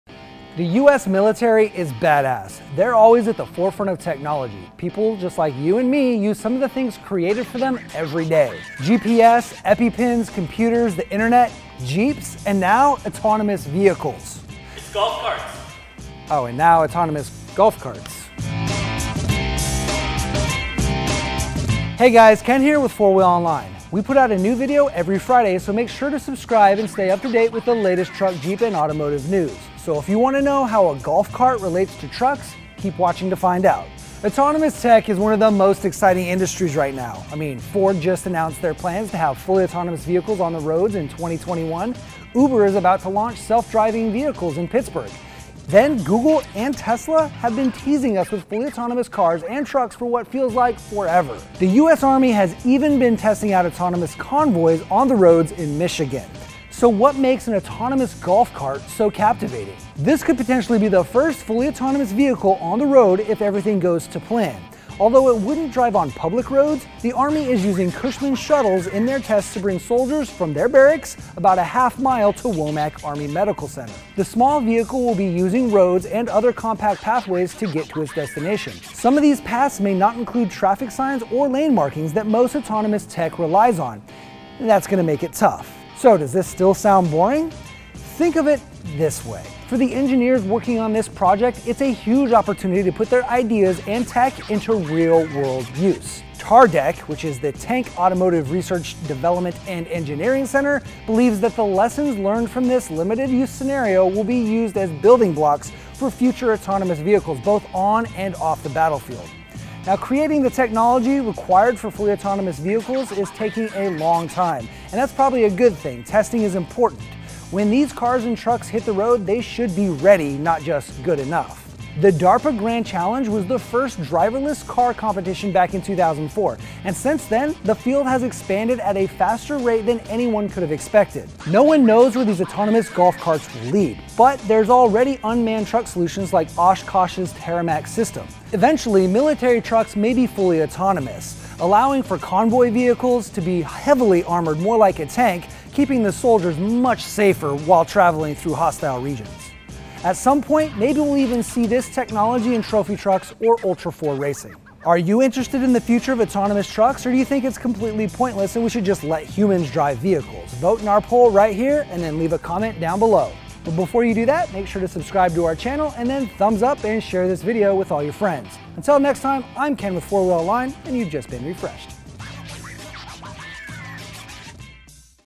The US Army’s First Autonomous Vehicle is Going to be... a Golf Cart?
Who would have thought that a golf cart would be the first autonomous vehicle used by the military? Despite the surprise the advancements in autonomous tech that this may bring are huge. How long until the knowledge gained from an autonomous golf cart rolls over to autonomous military trucks?

Transcript:
The US military is badass. They’re always at the forefront of technology. People just like you and me use some of the things they created for them every day.

GPS, Epipens, Computers, the internet, JEEPS and now Autonomous Vehicles.

Off-camera: It’s golf carts

Oh, and now autonomous golf carts?

Hey guys, Ken here with 4WheelOnline. So if you want to know how a golf cart relates to trucks, keep watching to find out.

Autonomous tech is one of most exciting industries right now. I mean, Ford just announced their plans to have fully autonomous vehicles on the roads in 2021, Uber is about to launch self-driving vehicles in Pittsburgh, then Google and Tesla have been teasing us with fully autonomous cars, and trucks, for what feels like forever.

The US Army has even been testing out autonomous convoys on the roads in Michigan.

So what makes an autonomous golf cart so captivating?

This could potentially be the first fully autonomous vehicle on the road if everything goes to plan. Although it wouldn’t drive on public roads, the Army is using Cushman Shuttles in their tests to bring soldiers from their barracks about a half mile to Womack Army Medical Center

The small vehicle will be using roads and other compact pathways to get to its destination. Some of these paths may not include traffic signs or lane markings that most autonomous tech relies on. That’s going to make it tough.

Does it still sound boring? Think of it this way: For the engineers working on this project this is a huge opportunity to put their ideas and tech into real world use.

TARDEC which is the Tank Automotive Research, Development and Engineering Center believes that the lessons learned from this limited use scenario will be used as building blocks for future autonomous vehicles both on and off the battlefield.

Now, creating the technology required for fully autonomous vehicles is taking a long time, and that’s probably a good thing. Testing is important. When these cars and trucks hit the road they should be ready, not just “good enough”

The DARPA Grand Challenge was the first driverless car competition back in 2004, and since then the field has expanded at a faster rate than anyone could have predicted.

No one knows where these autonomous golf carts will lead but there’s already unmanned truck solutions like Oshkosh’s TerraMax system. Eventually military trucks may be fully autonomous allowing for convoy vehicles to be heavily armored more like a tank. Keeping the soldiers much safer while travelling through hostile regions.. At some point maybe we’ll see the tech in trophy trucks or ultra4 racing.

Are you interested in the future of autonomous trucks or do you think it’s pointless and we should just let humans drive vehicles.

Until next time, I’m Ken with 4WheelOnline and you’ve just been refreshed.